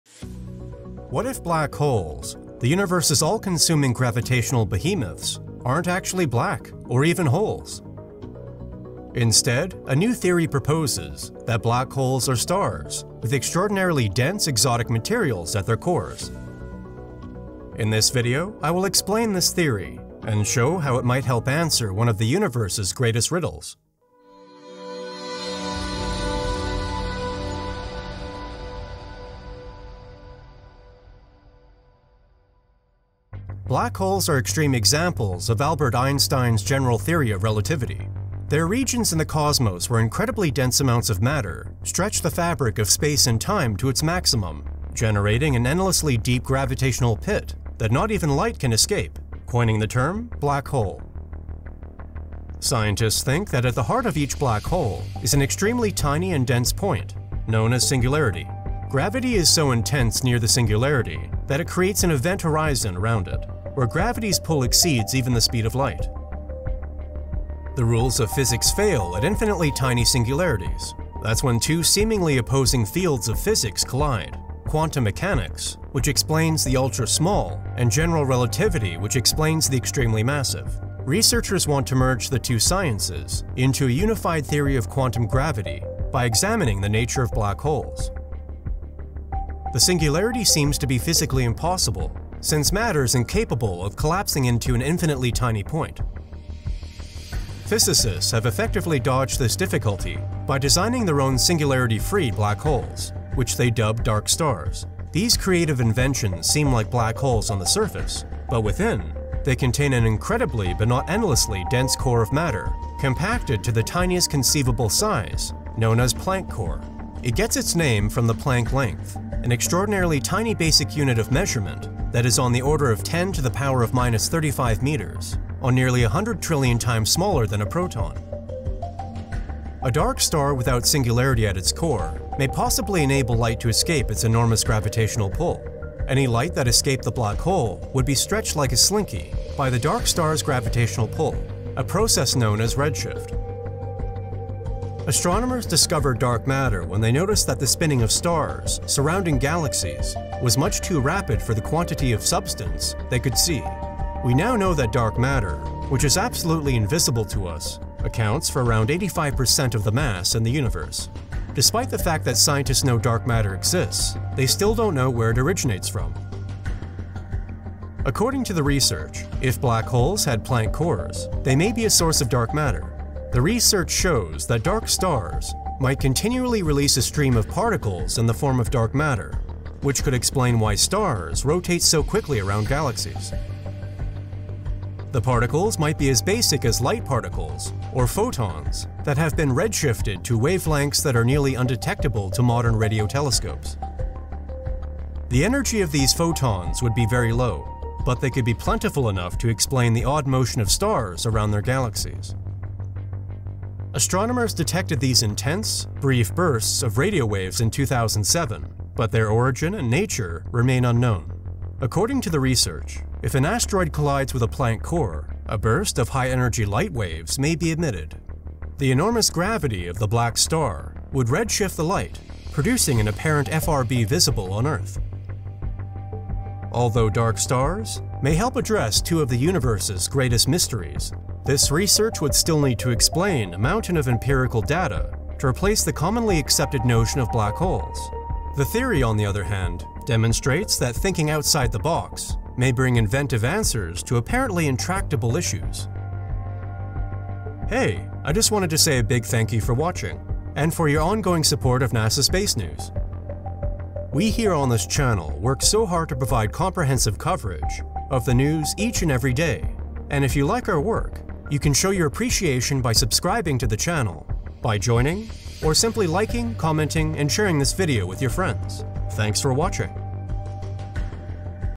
Are black holes actually black, or even holes?
What if black holes, the universe's all-consuming gravitational behemoths, aren't actually black—or even holes?

Instead, a new theory proposes that black holes are dark stars with extraordinarily dense, exotic materials at their cores.

In this video, I will explain this theory and show how it might help answer one of the universe's greatest riddles.